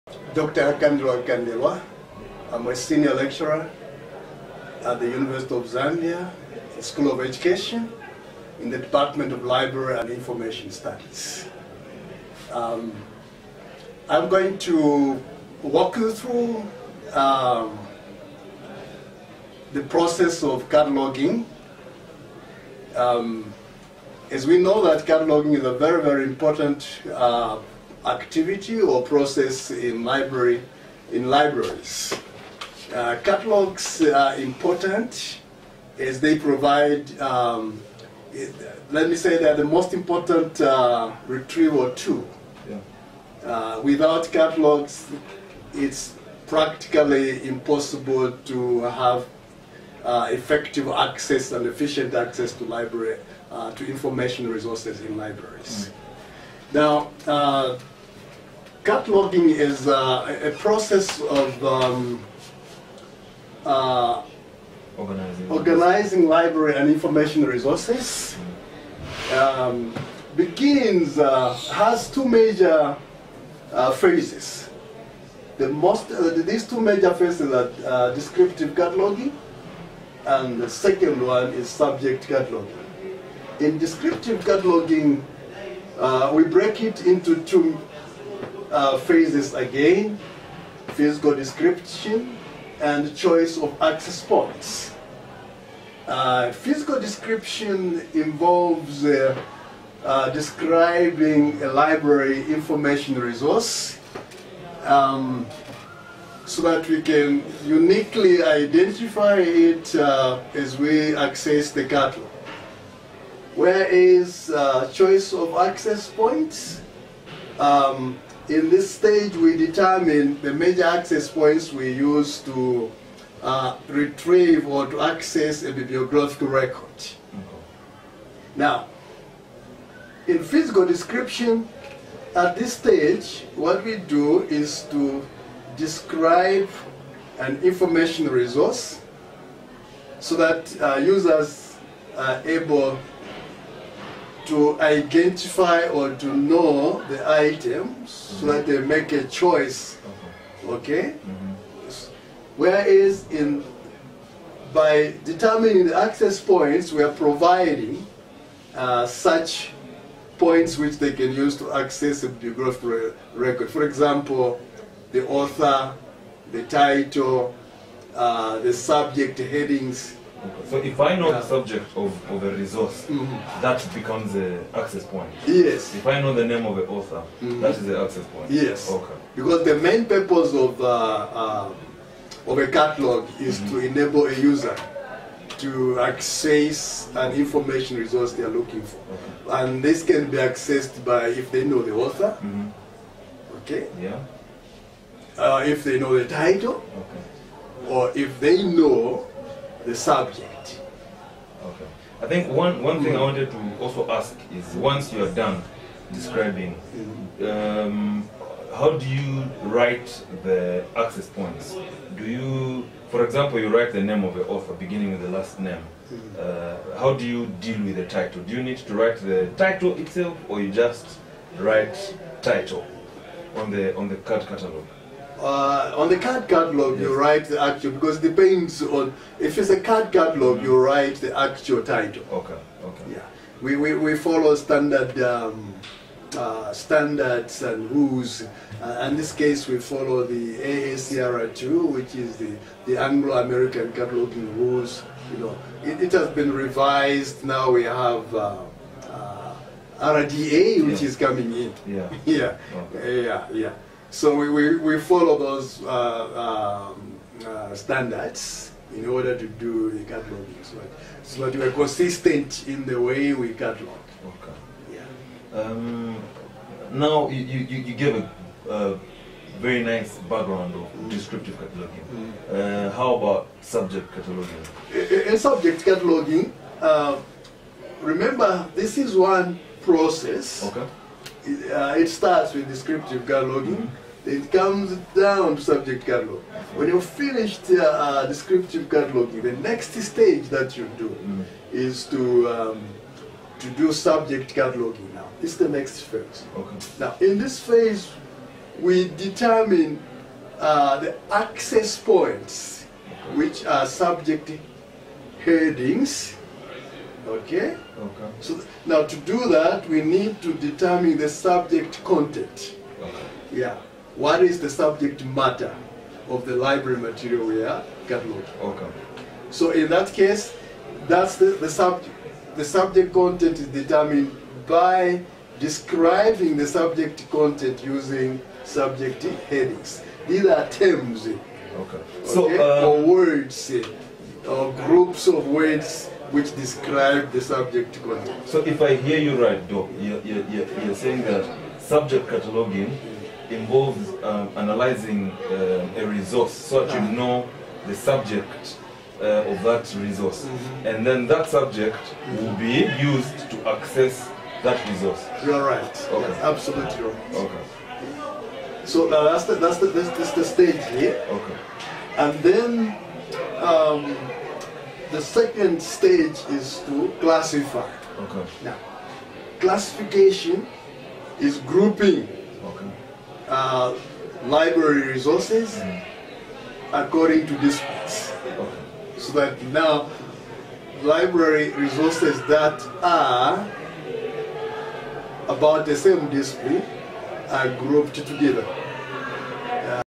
Information Processing: Cataloging and Classification - a summery
Dr Akakandelwa senior lecturer at the University of Zambia, in the department of library and information science discuss cataloging and classification.